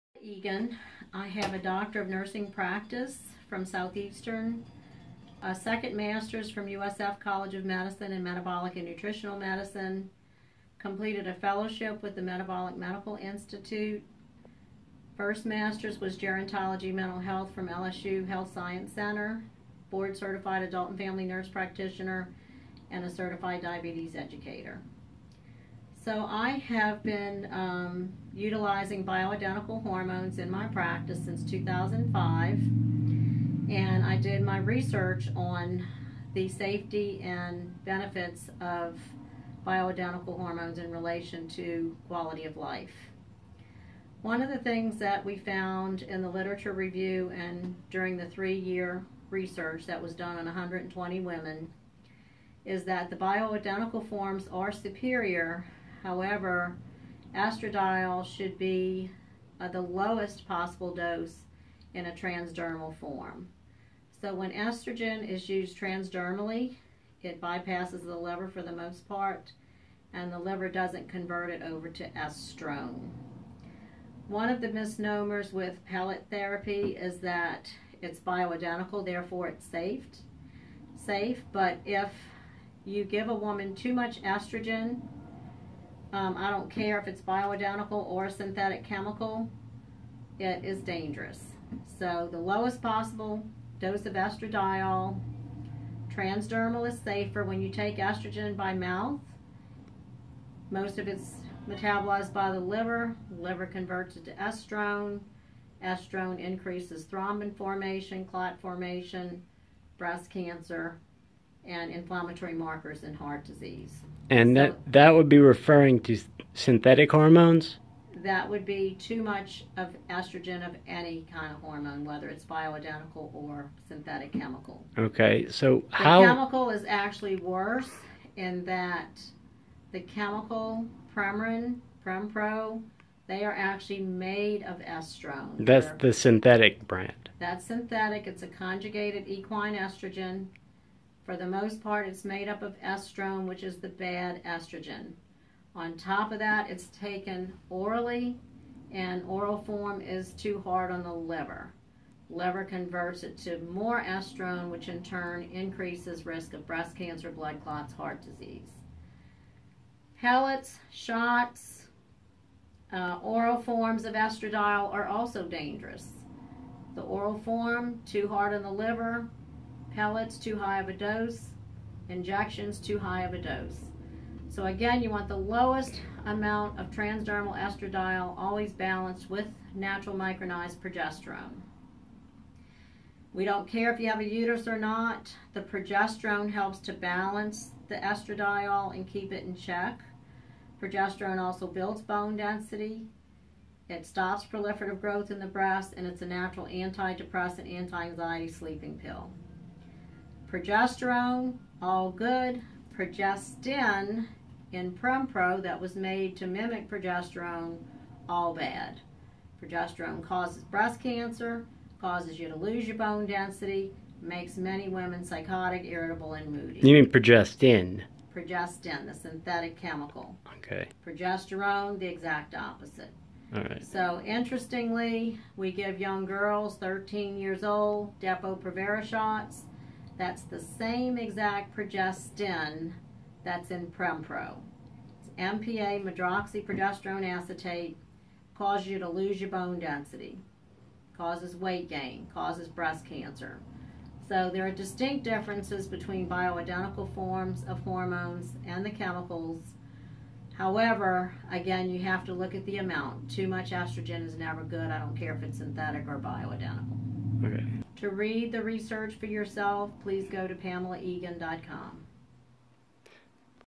Bioidentical Hormone Replacement Therapy (BHRT) Safer Than Synthetic HRT
Dr. Pamela Egan of the EGAN Wellness & Anti-Aging Clinic and EGAN Skin Care & Med Spa ) talks about new research she conducted in conjunction with Southeastern Louisiana University about Bioidentical Hormone Replacement Therapy (BHRT). The research suggests that contrary to popular belief, Bioidentical Hormones are more beneficial and pose fewer risks than traditional synthetic hormones to women undergoing HRT treatment.